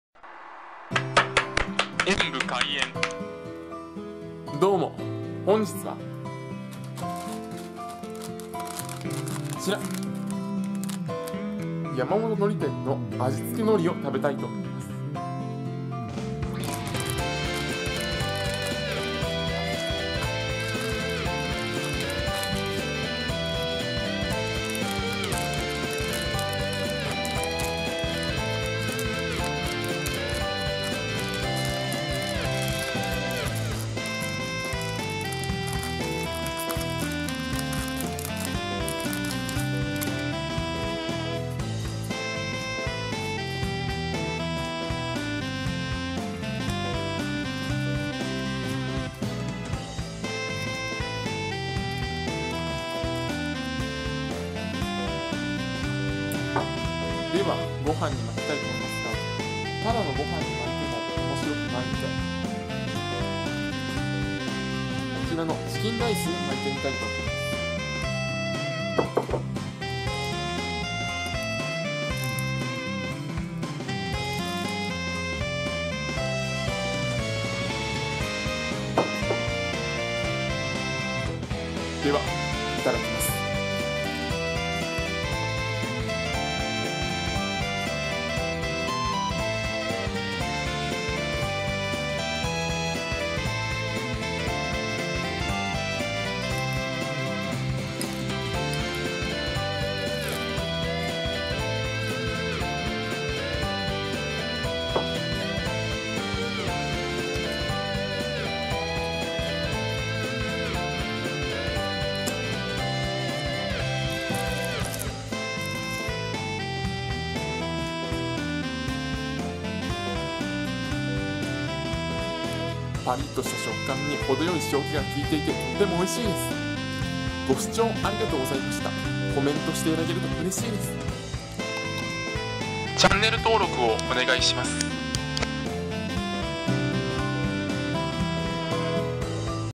山本海苔店 味付けのりを食べてみた
山本海苔店　味付け海苔を食べてみました。
パリパリと程良い塩気がいい！！